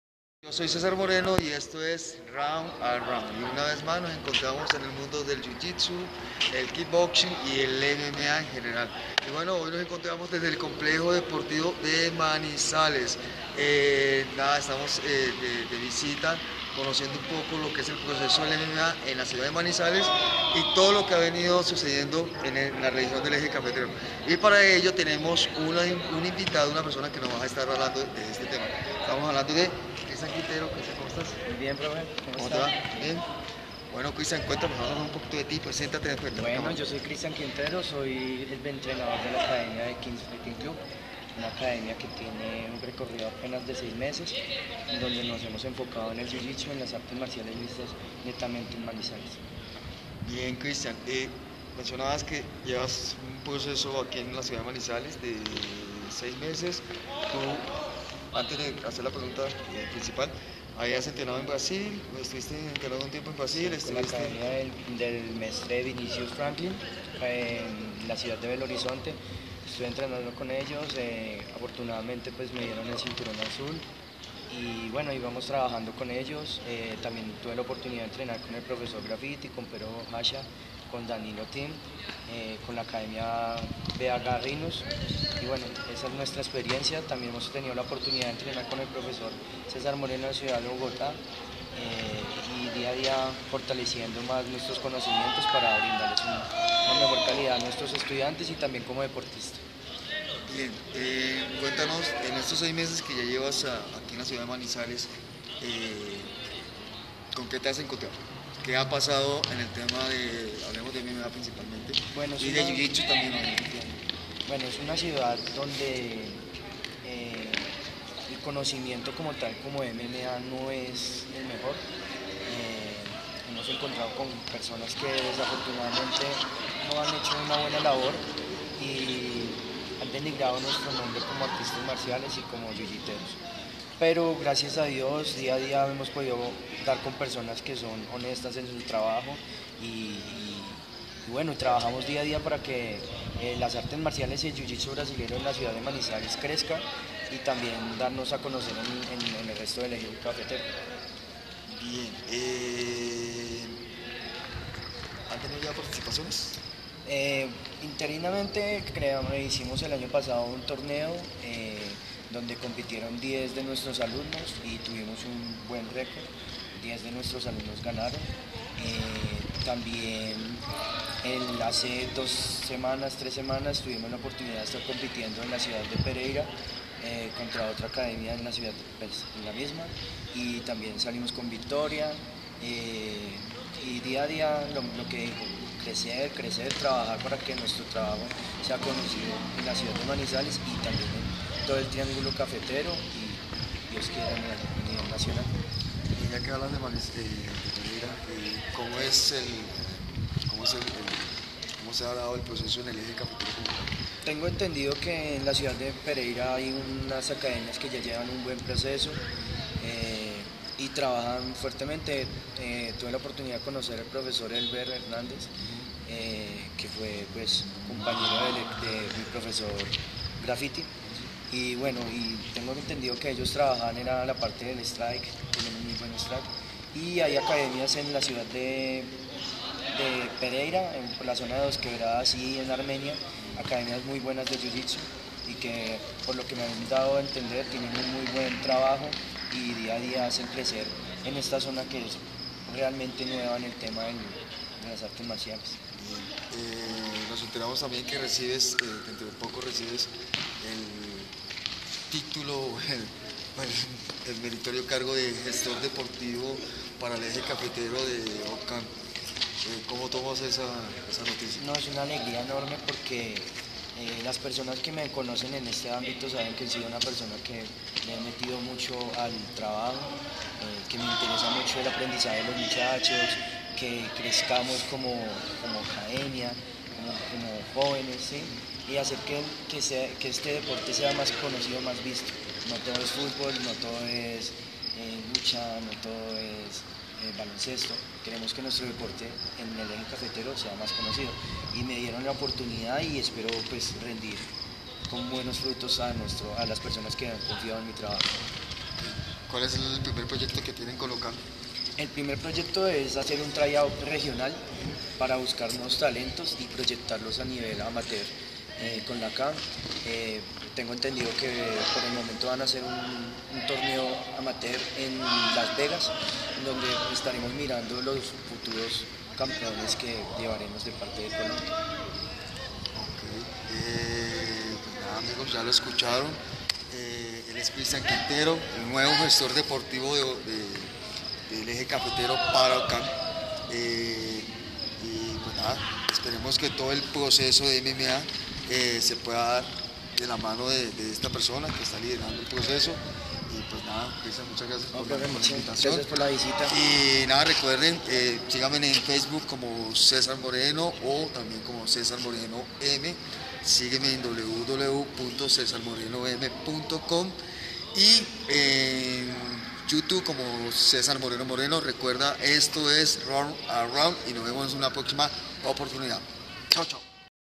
El MMA DE OCAMM LLEGA A MANIZALES - Entrevista a Cristian Quintero
Acabamos de terminar la jornada de ranking para el nacional de MMA OCAMM en Bogota y.